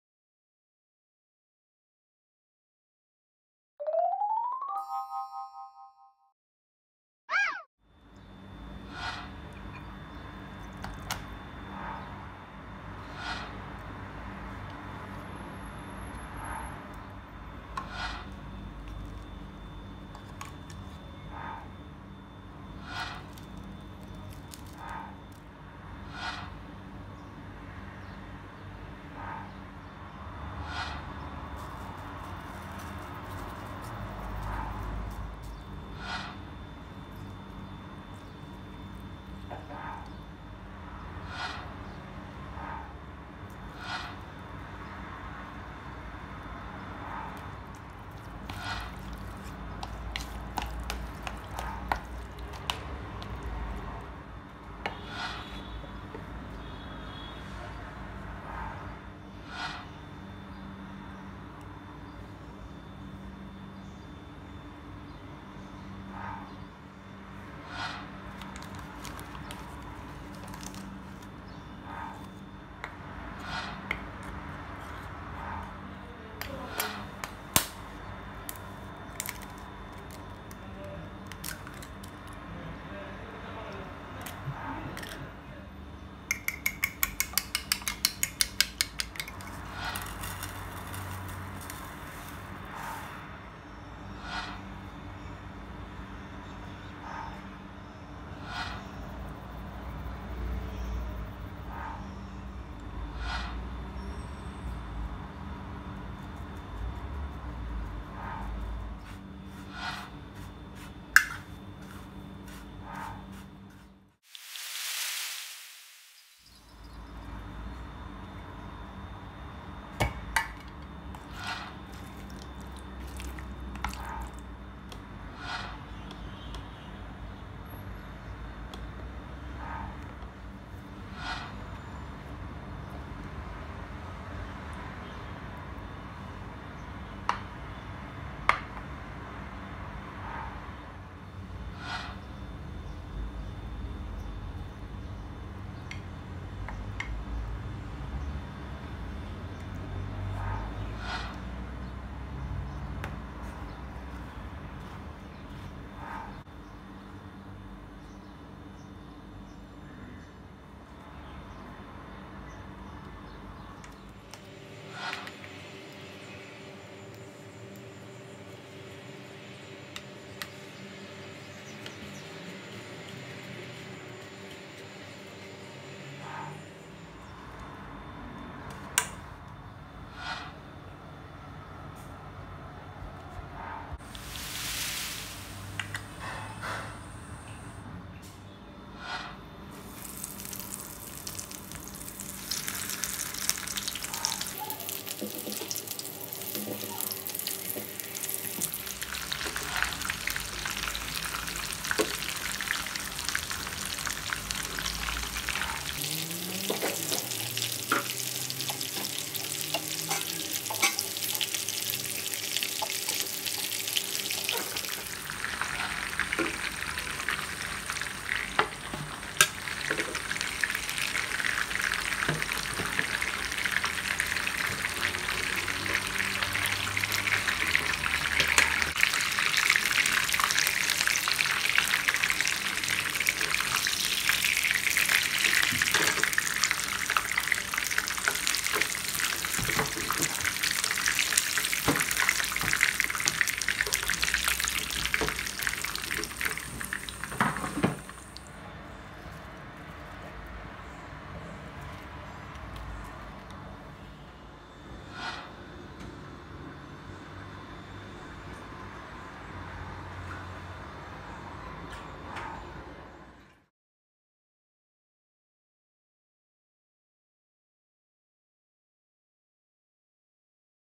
Chicken nuggets || easy and tasty || ifthar recipe
This recipe is finger liking 😋😋😋very very tasty and easy do try this recipe and comment me

Chicken nuggets

Ingredients

Red chilly powder -1/2 sp
Pepper powder -1/2 sp
Ginger garlic paste -1 sp
Mixed herbs-1/2 sp
Lemon -1/2

For coating

Maida-3 tsp
Cornflour-1.1/2 sp

Egg-1

Cornflakes crushed-1/2 cup
Bread crumbs -3/4 cup
Little salt
Mixed herbs